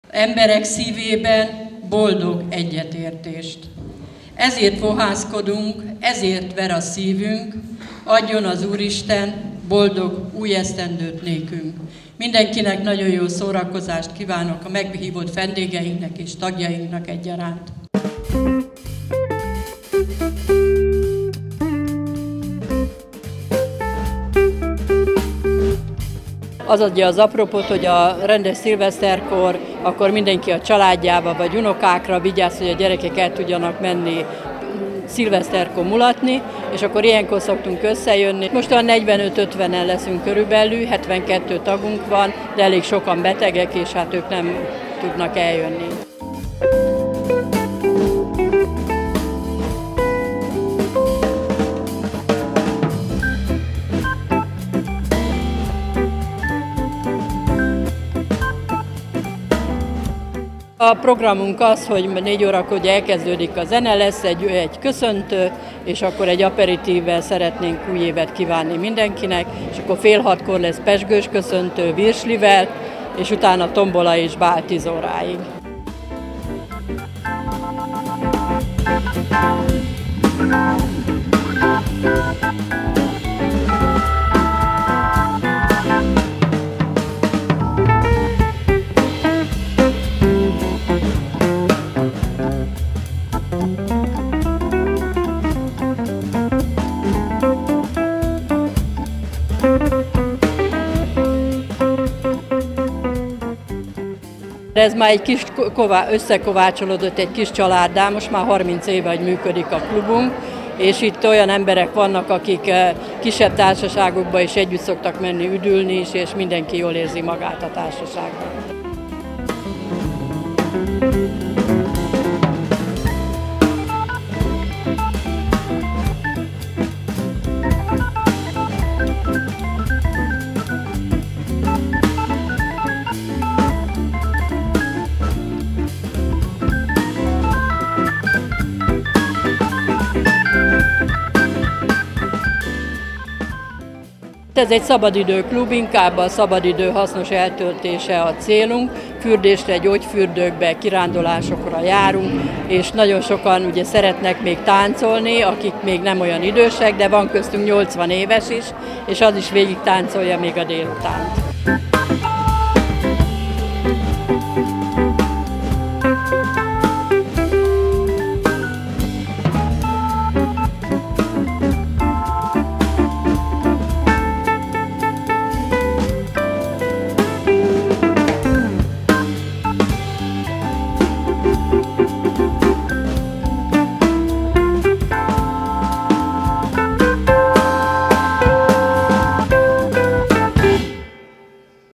Nyugdijas pótszilveszter - Tolna 20190108
Pótszilvesztert tartottak a Tolnai Nyugdíjas Szabadidő Klub tagjai hétfőn délután! A helyi lovardában a 72 fővel rendelkező közösség több mint 50 tagja köszöntötte együtt 2019-et. Ennek egyébként a Tolnai Nyugdíjas Szabadidő Klubnál már hagyománya van. Mivel közös szilvesztert nem szoktak rendezni, ezért év elején kerül megrendezésre az esztendőben az első közös zenés rendezvényük.